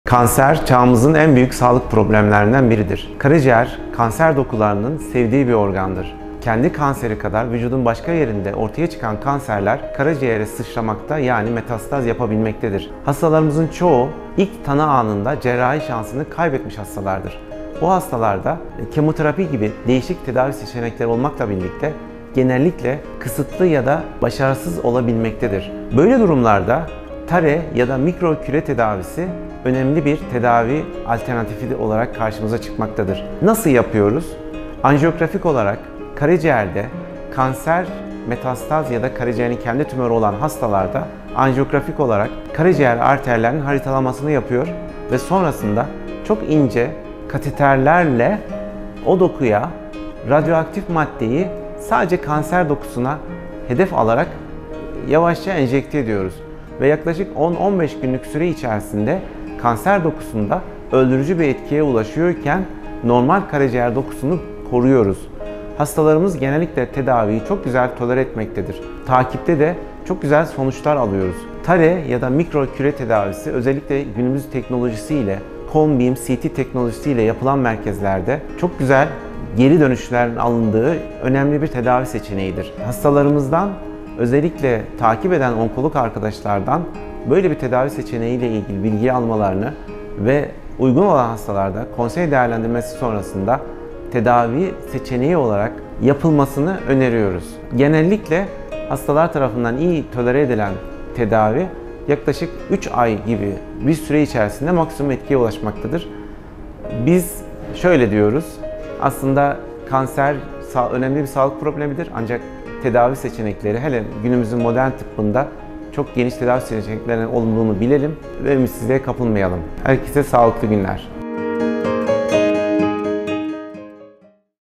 Karaciğer Kanserlerinde Mikro Küre Tedavisi - Girişimsel Radyoloji Prof. Dr. Akif Şirikçi
Medical Point Gaziantep Hastanesi Girişimsel Radyoloji Bölümünden Prof. Dr. Akif Şirikçi, karaciğer kanserlerinde mikroküre tedavisi hakkında bilgilendiriyor.